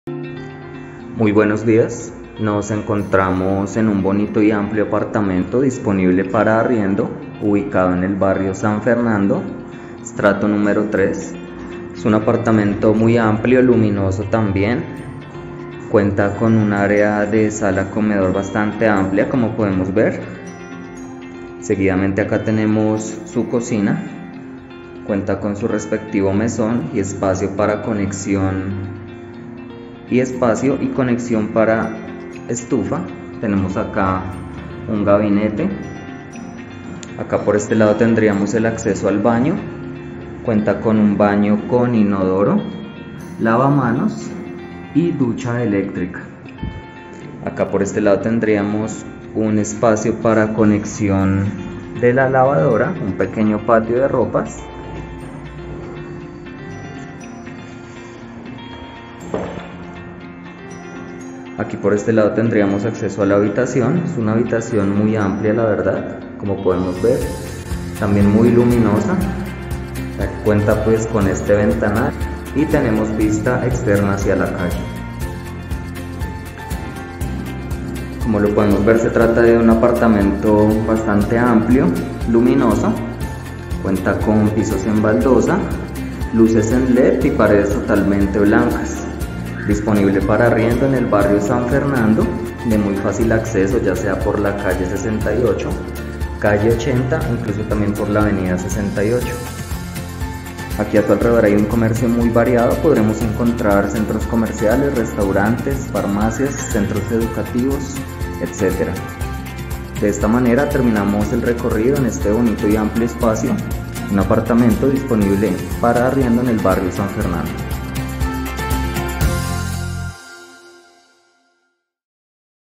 ALQUILER DE FINCAS
Alquilo apartaestudio en el barrio San Fernando, tercer piso, bien iluminado, con vistas al exterior. Consta de una habitacion amplia, 1 baño, cocina , estufa con horno, sala comedor, patio de ropas. Acceso facil a transporte publico, zona muy comercial y cerca a parques y centros comerciales. Se aceptan mascotas.